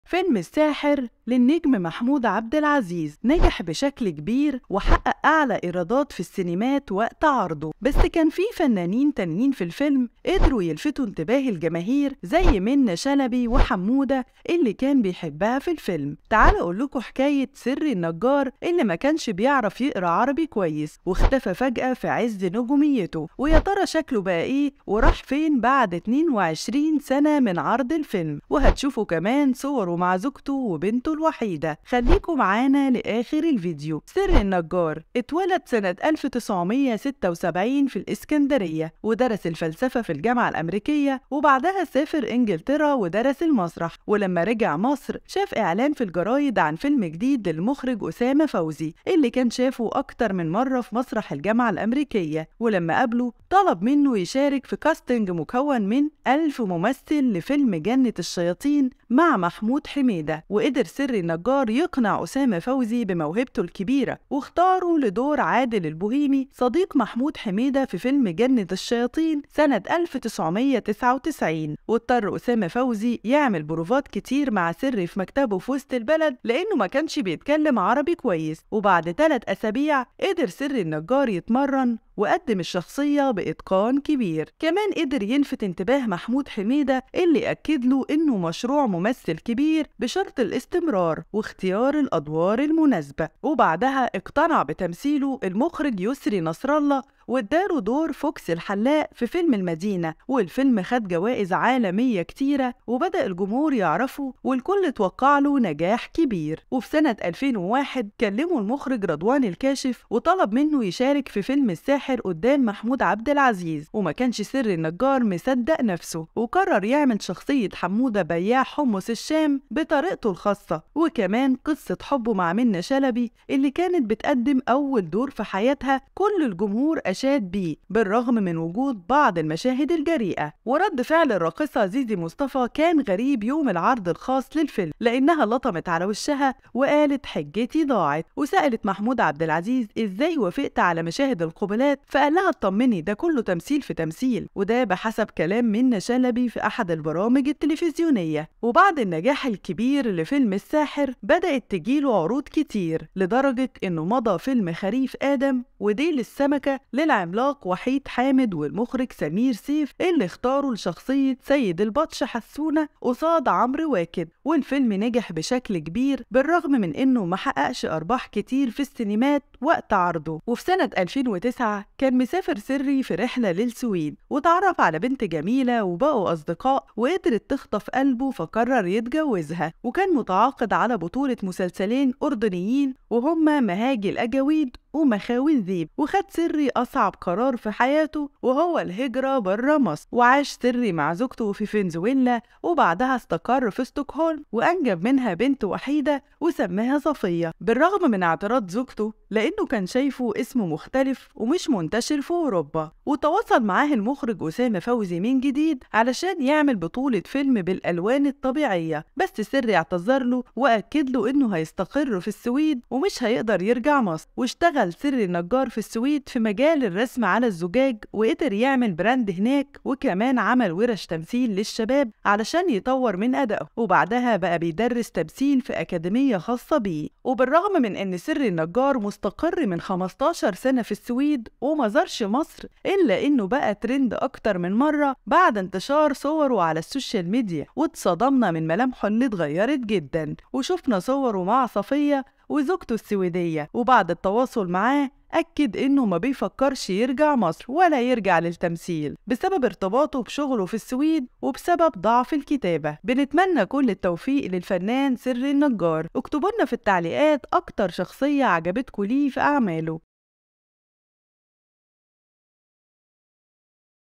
أم منة شلبي قالت لمحمود عبد العزيز إزاي وافقت إن بنتي تعمل كدا.. حكاية حمودة بياع حمص الشام
أم منة شلبي قالت لمحمود عبد العزيز إزاي وافقت إن بنتي تعمل كدا.. حكاية حمودة بياع حمص الشام عنده بنت وحيدة ومش ناوي يرجع مصر تاني